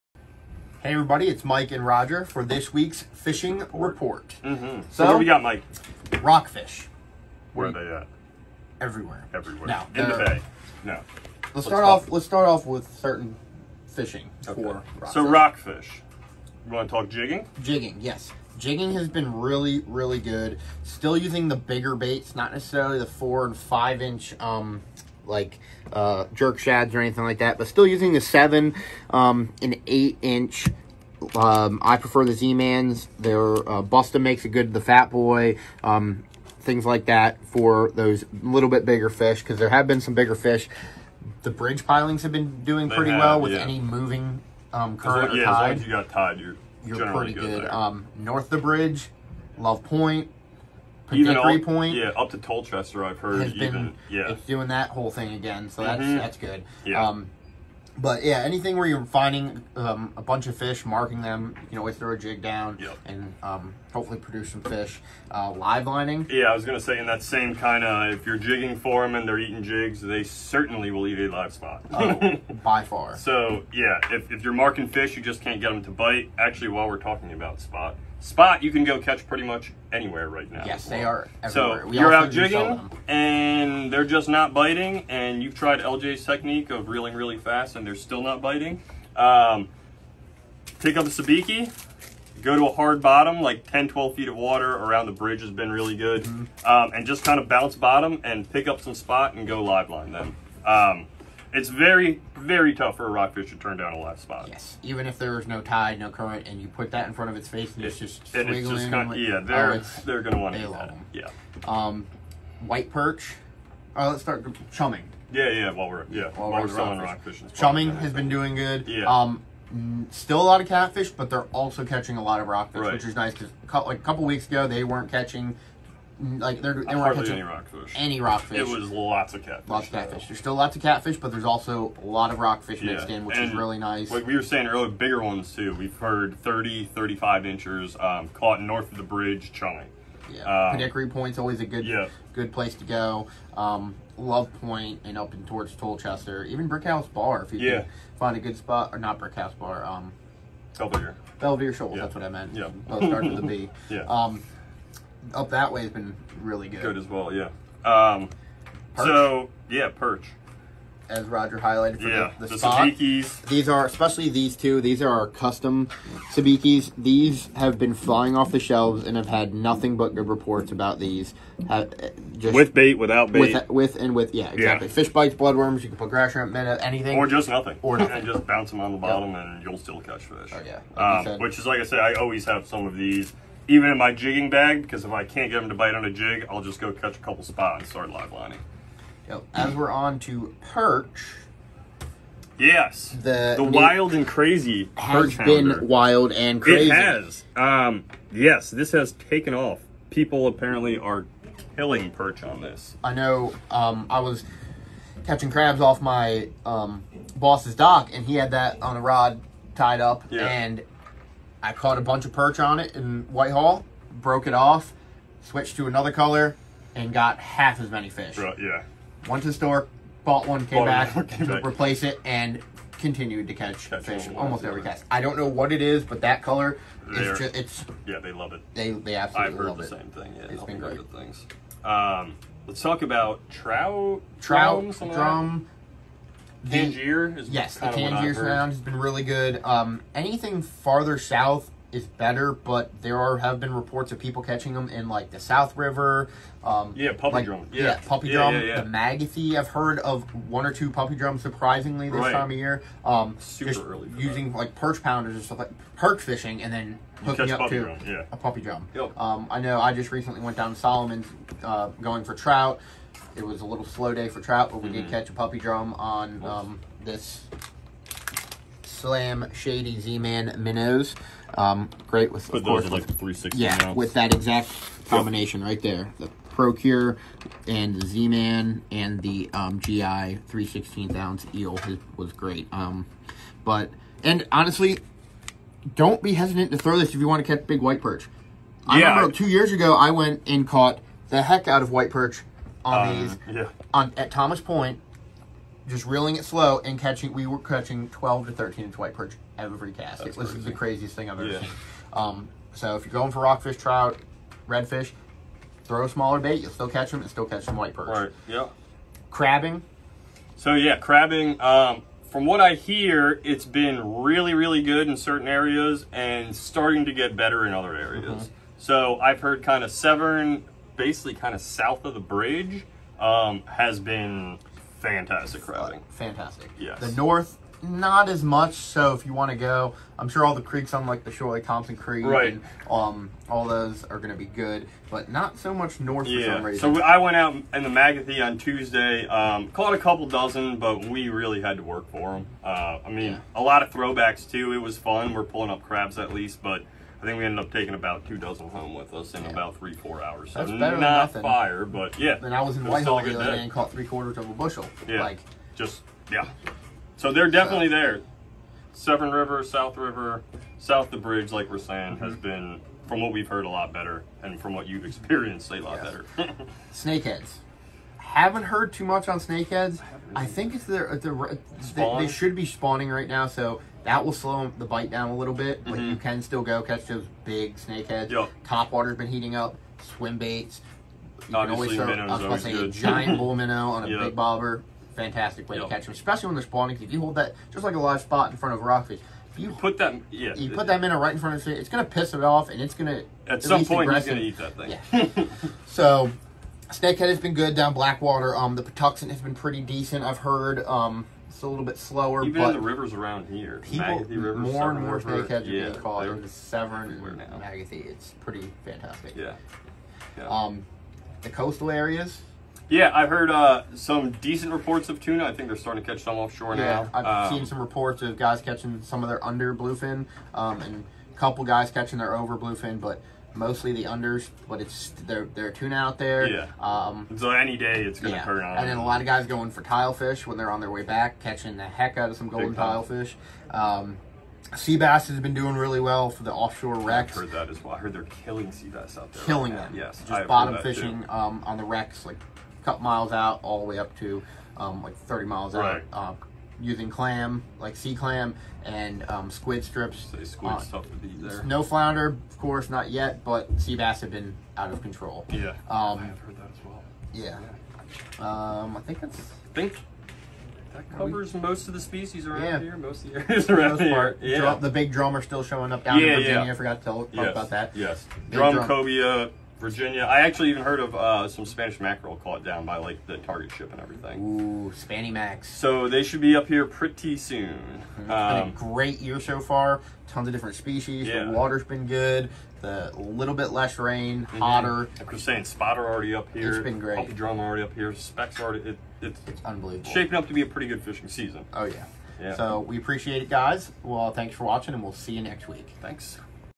Chesapeake Bay Fishing Report: June 4th 2021
Cover Image: Three generations of fishing! Anglers owner Charlie, his daughter and Anglers manager Anna, and Anna's daughter Ella caught perch using soft crab and catfish using fresh alewife!